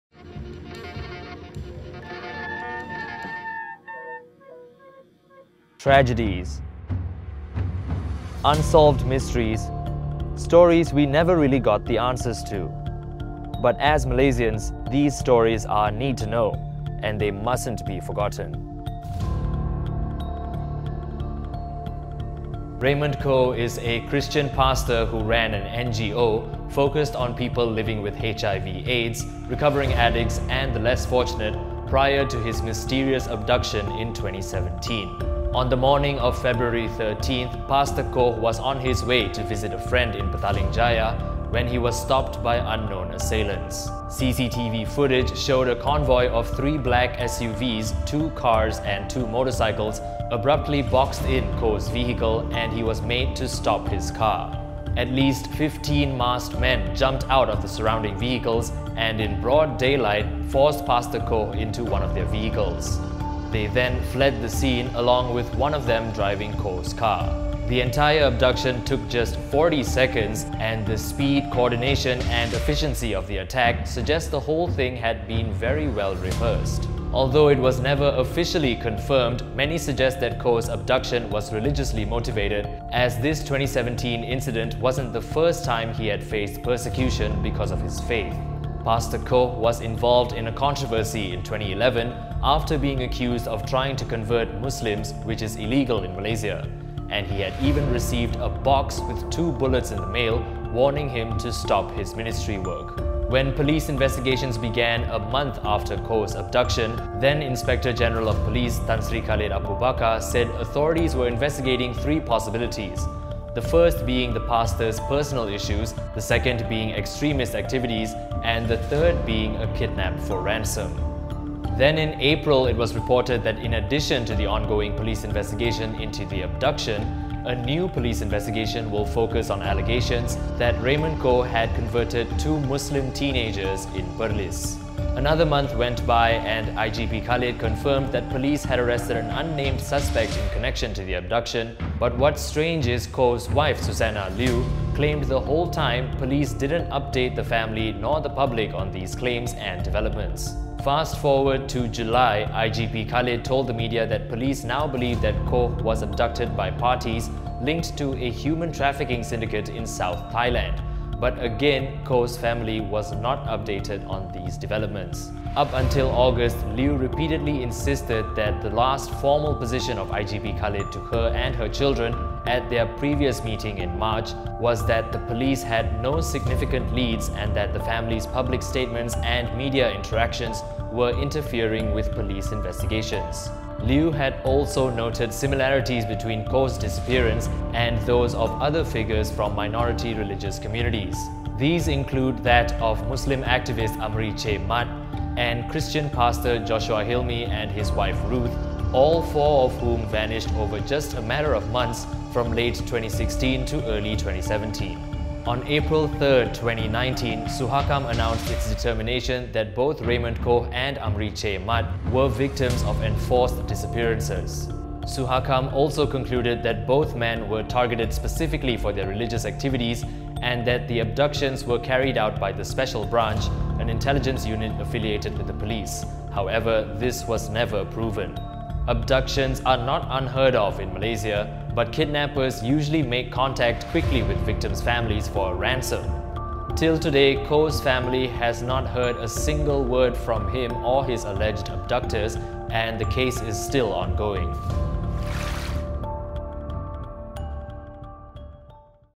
SYOK ENG | RememberThis: The Disappearance Of Pastor Raymond Koh
Enforced disappearances – a topic of conversation that people can only talk about, but when it comes to providing proof, there’s usually never any public knowledge on whether they actually happened.

The 2017 abduction of Pastor Raymond Koh is only one of many such cases in Malaysia, and as investigations into his disappearance continue, his family is still in the dark about his whereabouts.